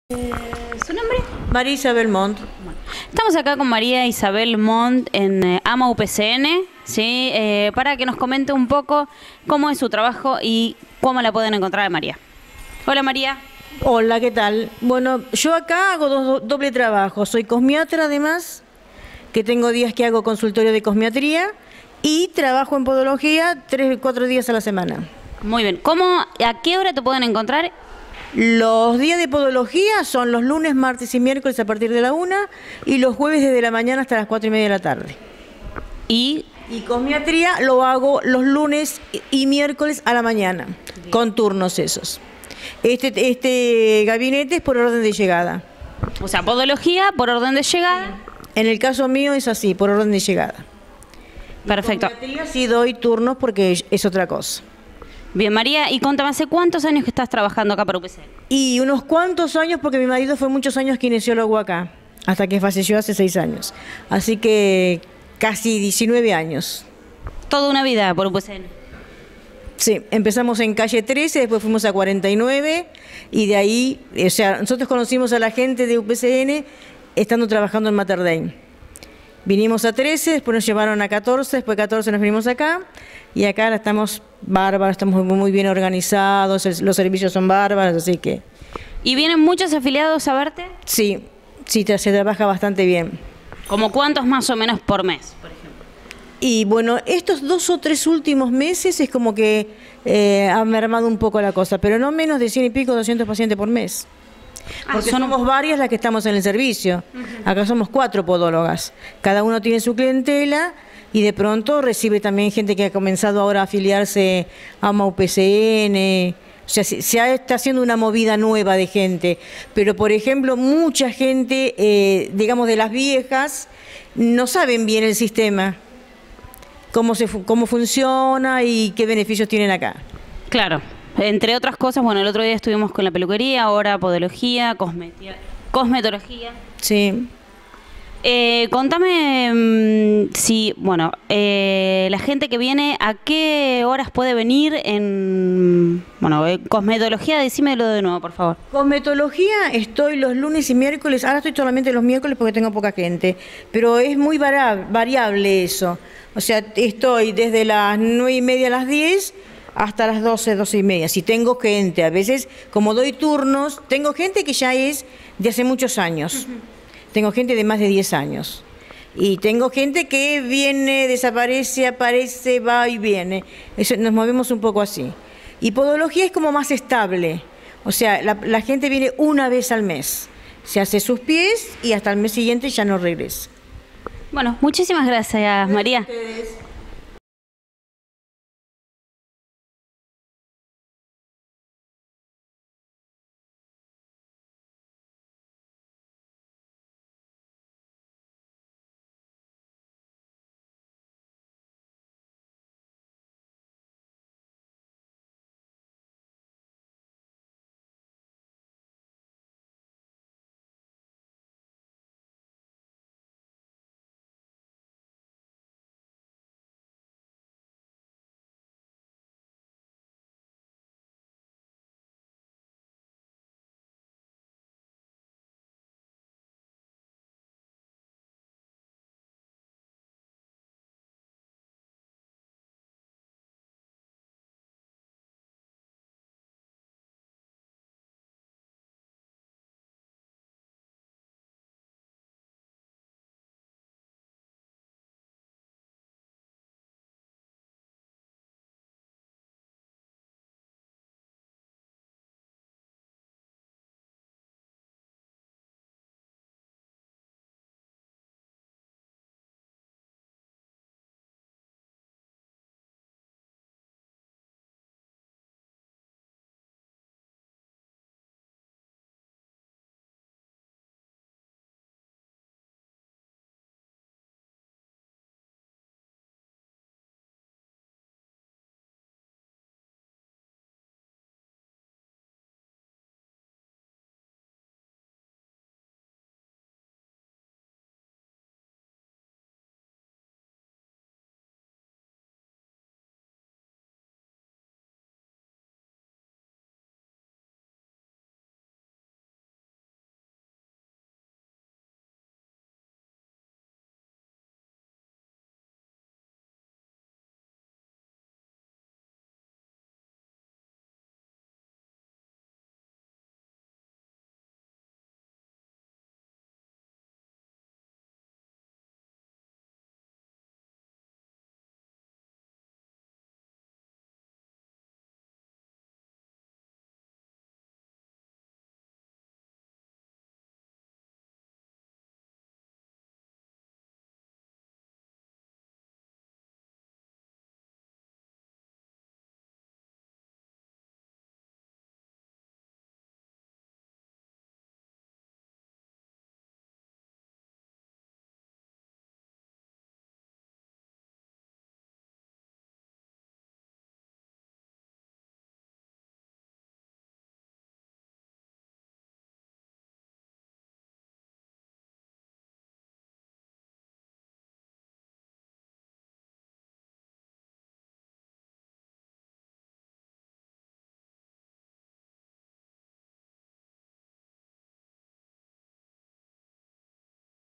MARIA ISABEL MONT PODOLOGA DE AMAUPCN
LA PODODOLOGA MARIA ISABEL MONT NOS CUENTA DIAS Y HORARIOS DE ATENCION